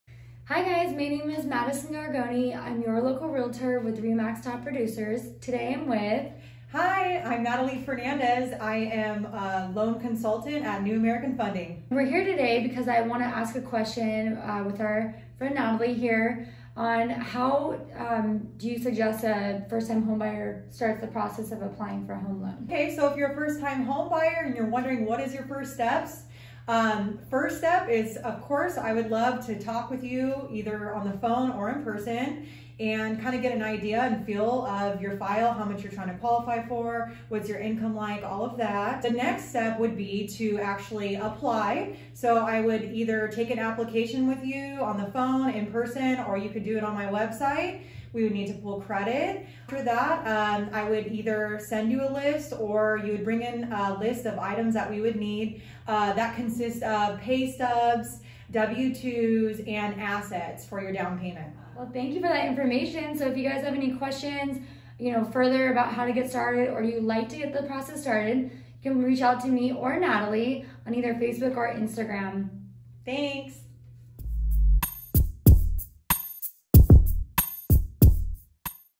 First Time Home Buyer - How to Get Started
Ready To Buy Your First Home But Not Sure Where To Start?

Before shopping for a home, first we need to know your BUDGET! Speaking with a lender or bank consultant is the first step in applying for a loan. Natalie with New American Funding shares these steps to get started:

- Set up a Consultation with a lender or bank by either phone or in person
- Apply for the loan either online or in person
- Submit required documentation including pay stubs, W2s, etc.

If you have any questions regarding the loan process and how to get started, please message me!

Madison Gorgone l Realtor
DRE# 02106767
RE/MAX Top Producers
VALUE, INTEGRITY, COMMITMENT
“My #1 Priority is to SERVE With Your Best Interest at 💛”